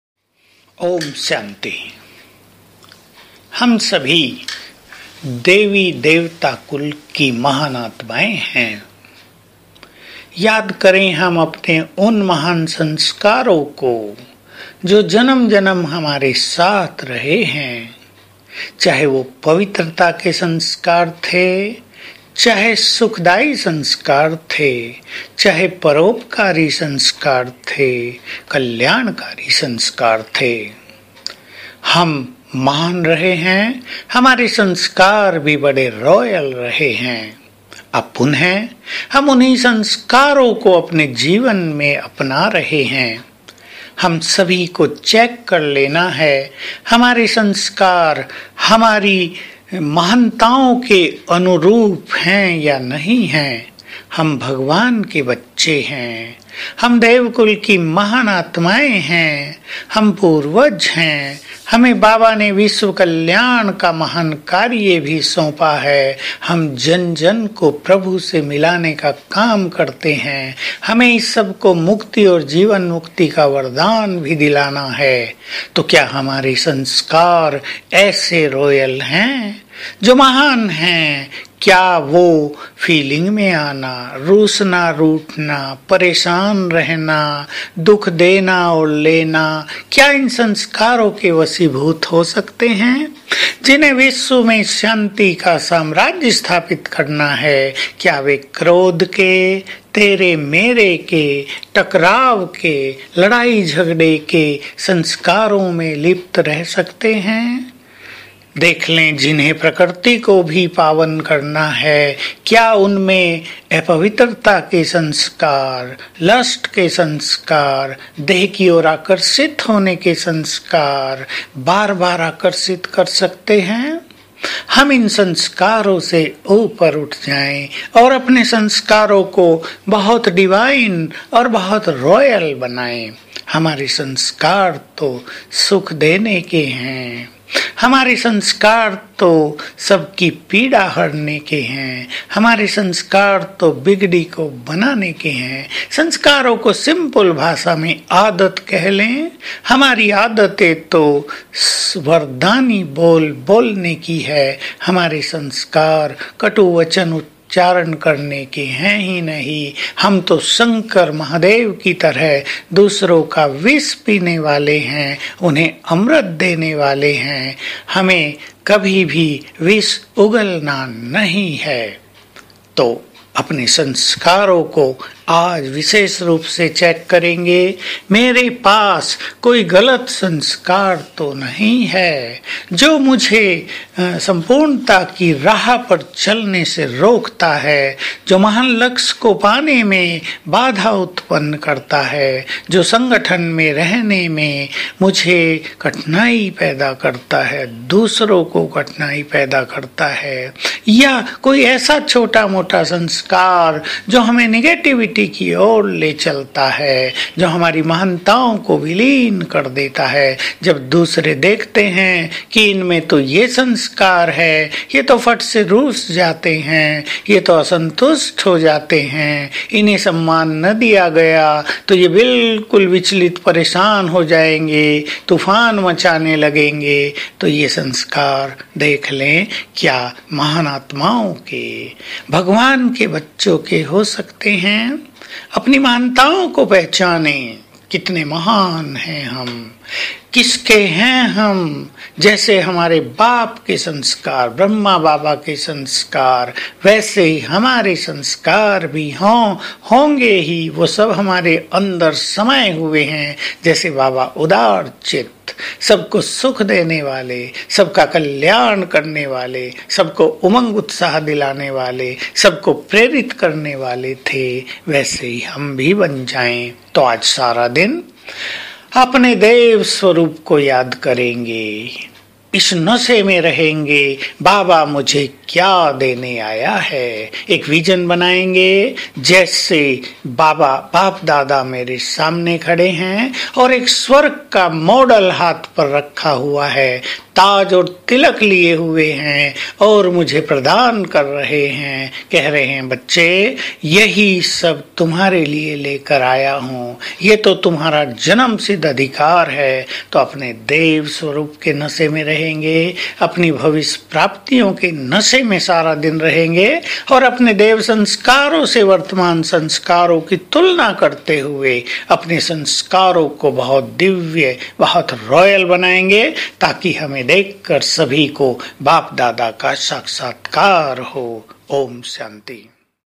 देवी-देवता कुल के दिव्य-रॉयल संस्कारो को इमर्ज करने की बेहद सहजविधि&अभ्यास.. By माननीयB.K.सूरज भाईजी
ये वीडियो है--पूरे विश्व की मनुष्य आत्माओं को लौकिक से अलौकिक बनाने की बेहद ईश्वरीय सेवार्थ निमित्त है।

आडियो वक्ता है - माननीय ब्रह्माकुमार सूरज भाईजी (मधुवन)की। (ज्ञानसरोवर,राजिस्थान)

परमिशन- ईश्वरीय विश्वसेवा चैनल को अपनी सभी आडियो क्लीप डालने की परमिशन दी है, स्वयं भ्राता माननीय सूरज भाईजी ने, उनका निवास है ब्रह्माकुमारी ज्ञानसरोवर,माउण्ट आबू,राजिस्थान।।।